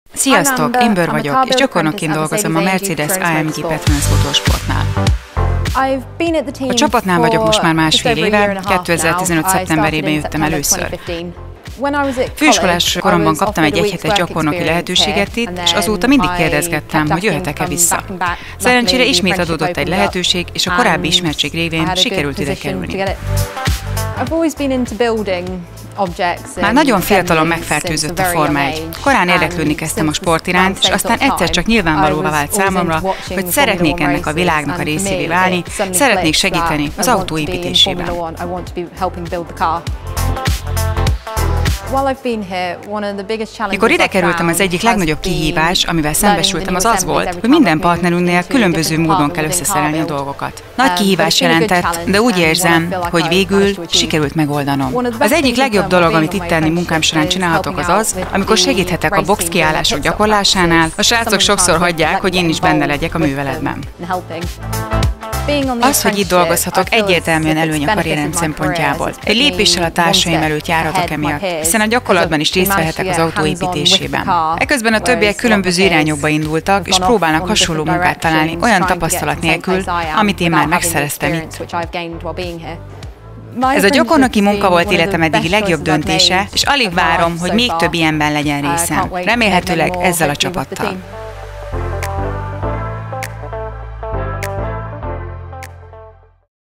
AFM április - A Mercedes legfiatalabb hölgyalkalmazottja
Betekintést nyújtunk a fiatal hölgyek munkájáról a Mercedes Forma-1-es bázisán.